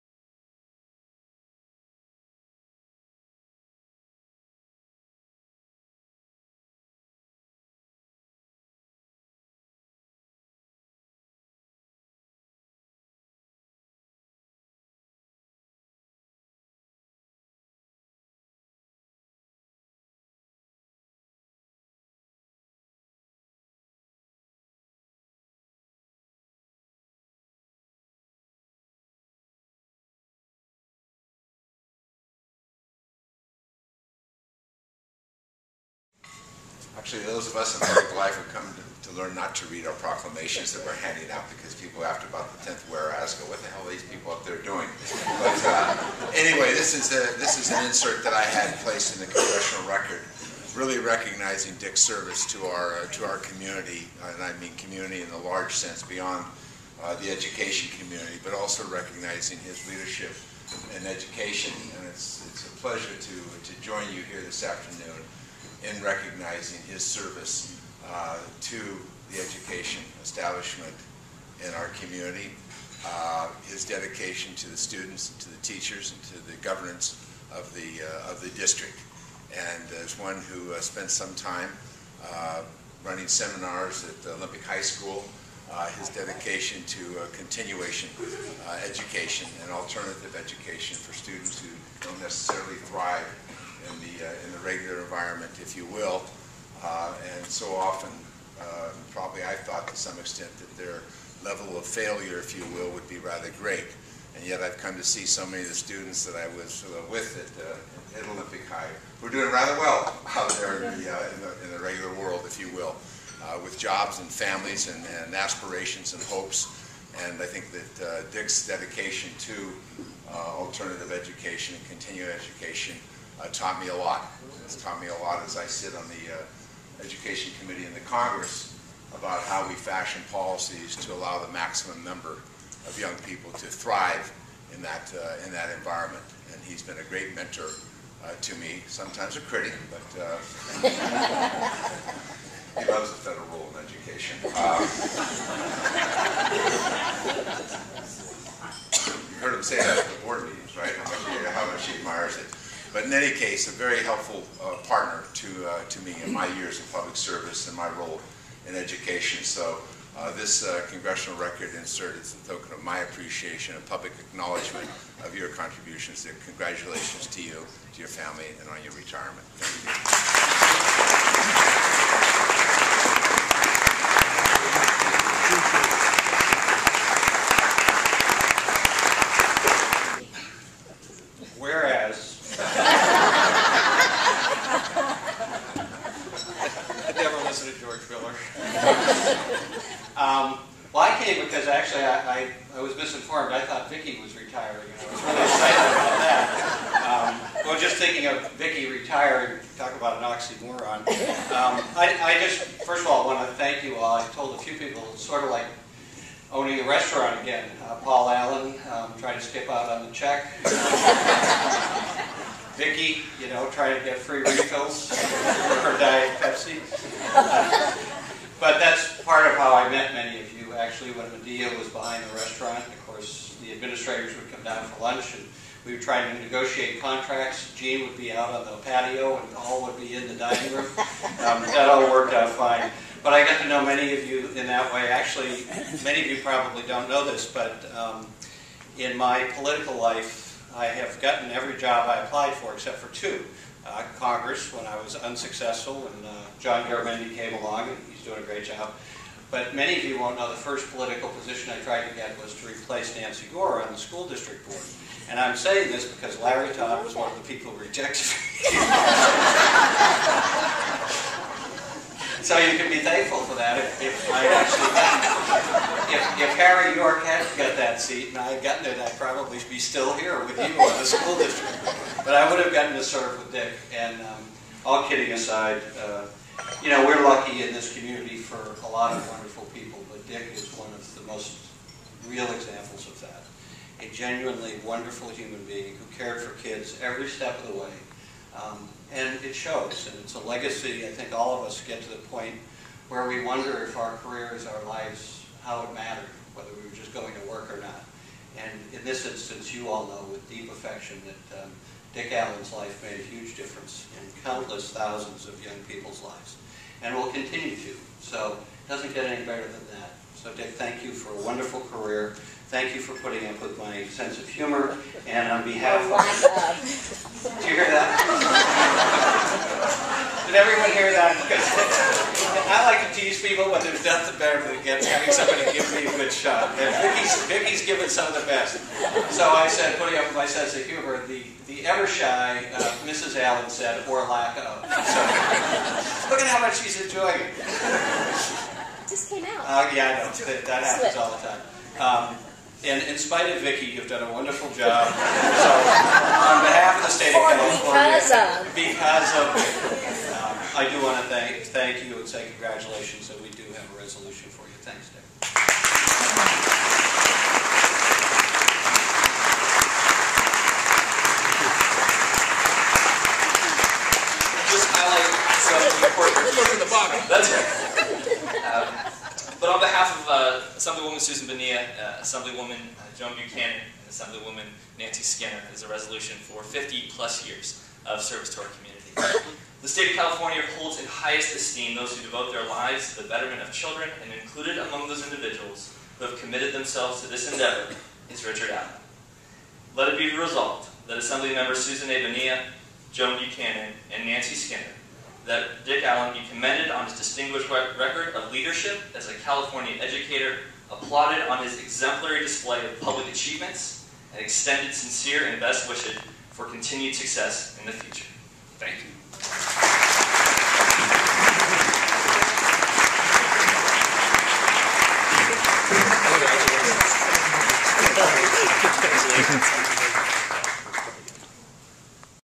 Richard Allen Retirement (1 of 6)
January 30, 2011. Contra Costa Country Club. Speakers in this video: Rep. George Miller (D-CA), CA Sen. Mark DeSaulnier